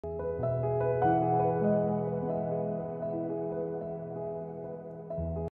It’s hard to get work done with cute lambs at your heels
You can’t get anything done around the barnyard now without a pack of lambs at your heels, but then again why would you want to get any work done when you could be playing with lambs?💗